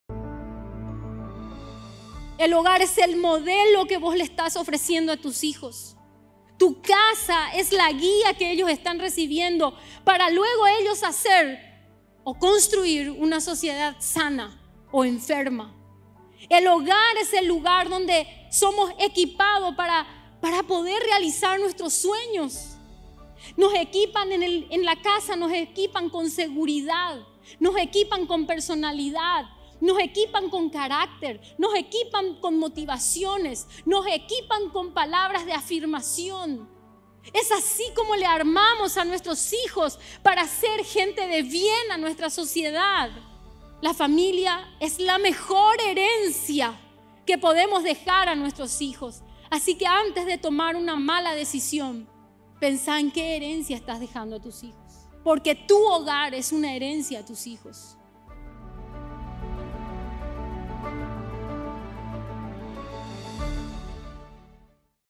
MDI251 Minuto de Impacto MQV - La mejor herencia para tus hijos
Nuestros hijos son el resultado de todo lo que han recibido en el hogar. Es allí donde son equipados para ser agentes de bien o de mal para nuestra sociedad.
Así que piensa hoy, ¿cuál es la mejor herencia que puedes dejarles? ¿Qué estás cultivando hoy en sus vidas?

➡️ Fragmento del mensaje "Olvidando el pasado, corrigiendo el futuro", de Liliam Medina de Agüero, compartido en el Encuentro de Mujeres Entre Amigas, a ser emitida este domingo 19 de junio, a las 10:00 de la mañana, por canal Trece.